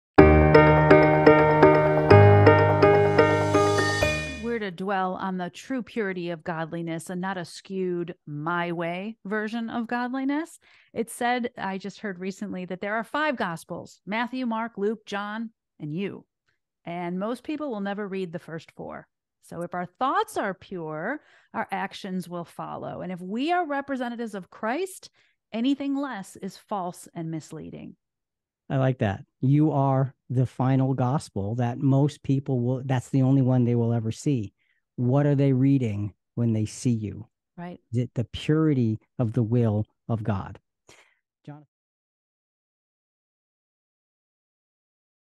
What Is The "Fifth" Gospel? CQ Podcast Highlight
Is there a "fifth" Gospel? How can we demonstrate true godliness in a way that everyone can understand?

We are to dwell on the true purity of godliness and not as skewed my way version of godliness. It said, I just heard recently that there are five gospels, Matthew, mark, Luke, John, and you. And most people will never read the first four. So if our thoughts are pure, our actions will follow. And if we are representatives of Christ, anything less is false and misleading.

I like that you are the final gospel that most people will, that's the only one they will ever see. What are they reading when they see you, right? Is it the purity of the will of God?

For more on lessons from Philippians listen to the full podcast

for more audio, video and Bible study materials.
--Follow us @cqbiblepodcast